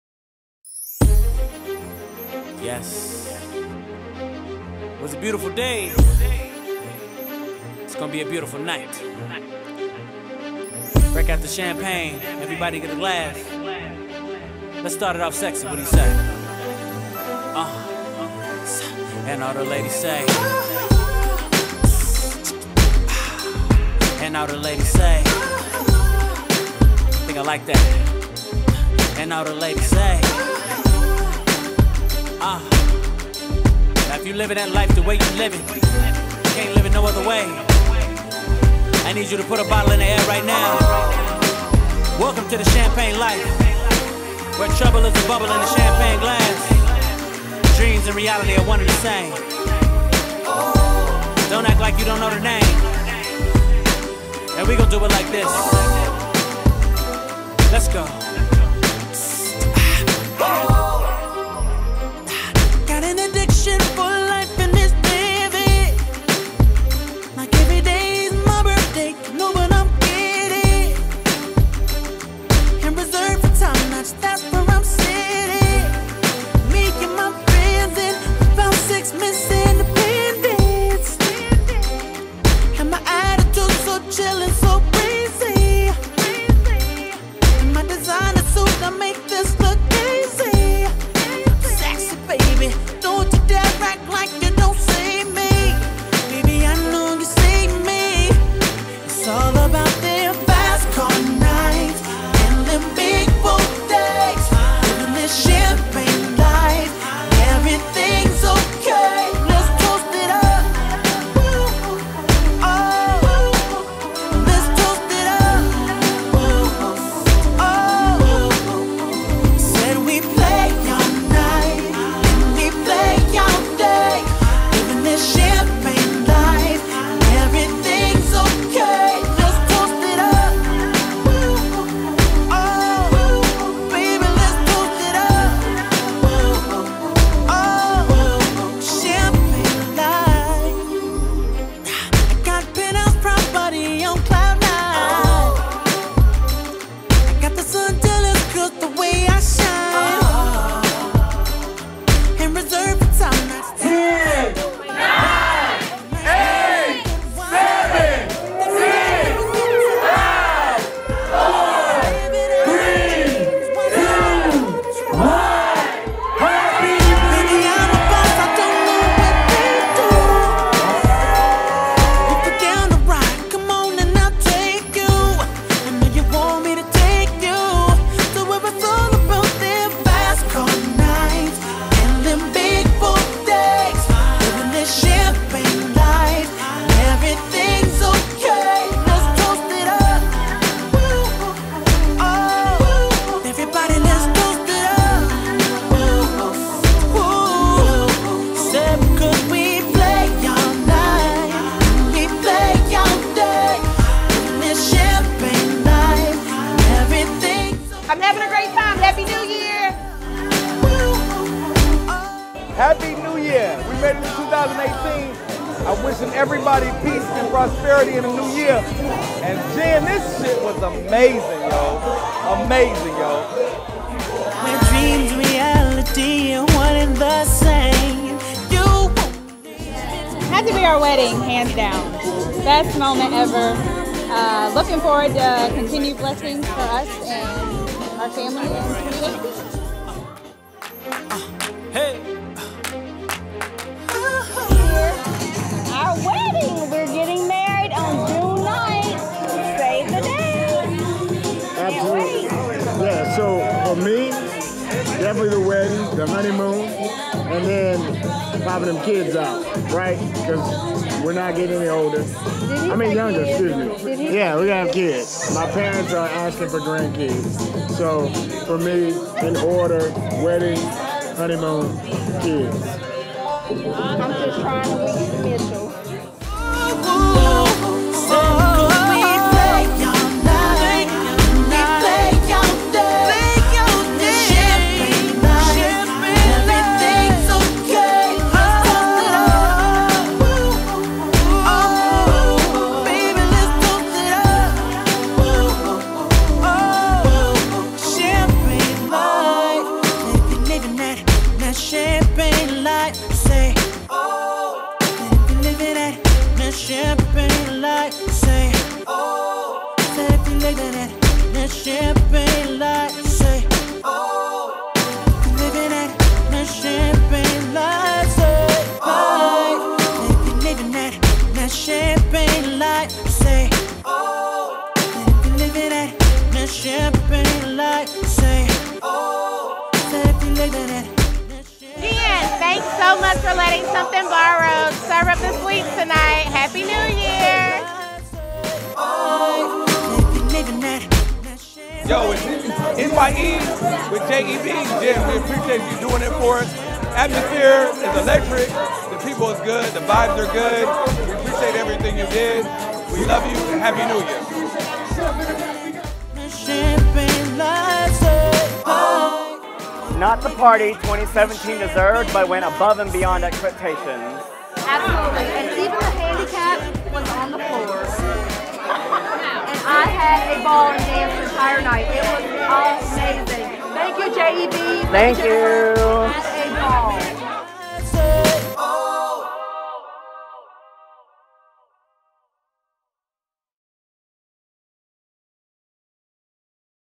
DOPEST NEW YEARS EVE PARTY!!!! | HOTEL | GREENSBORO, NC
Amazing NYE party Jennifer Bowden held and Katrena Wize Artography was honored to be a part of her special night! HAPPY NEW YEAR!!! We're talking food, Live mannequins, open Bar, and so much More. Liiiiiiivvvvvveeee! I love it!